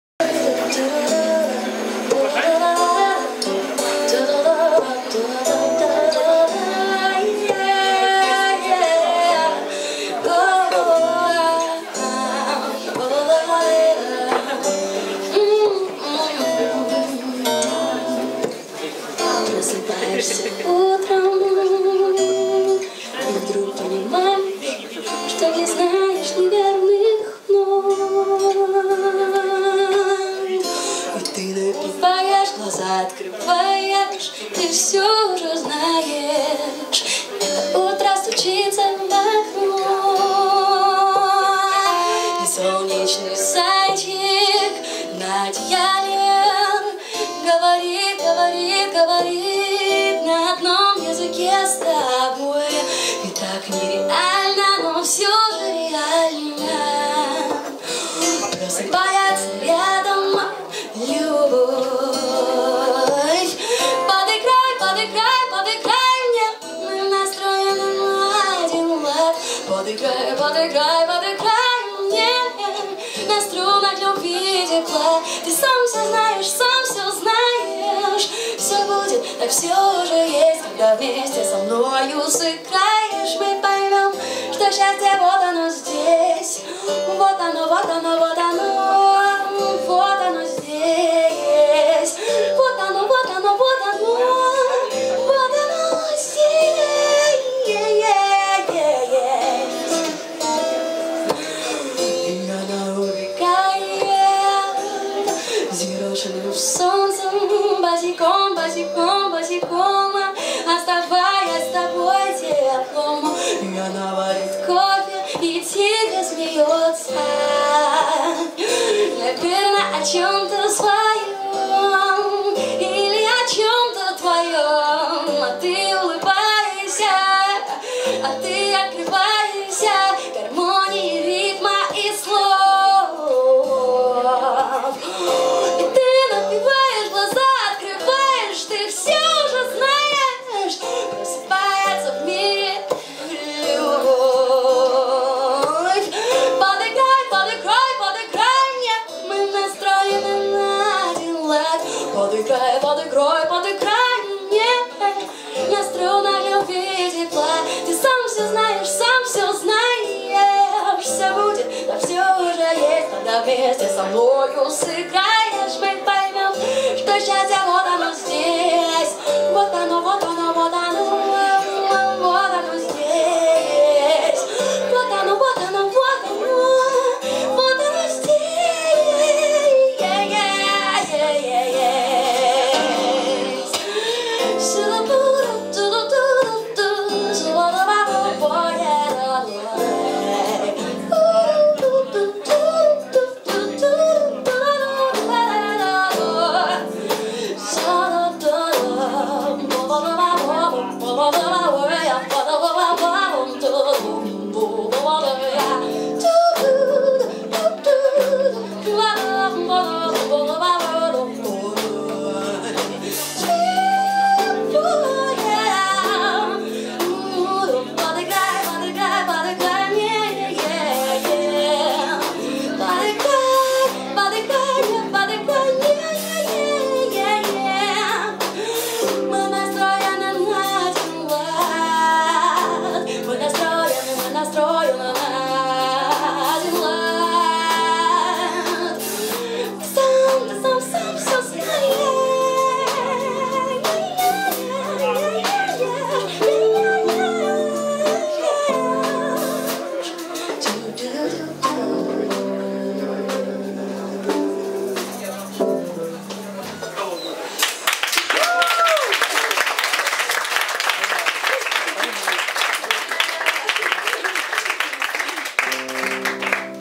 Squat cafe: Polina Daeven songs1_12.10.2011
В ходе Open Jam в Сквот-кафе необыкновенная Полина Даэвен исполняет авторские песни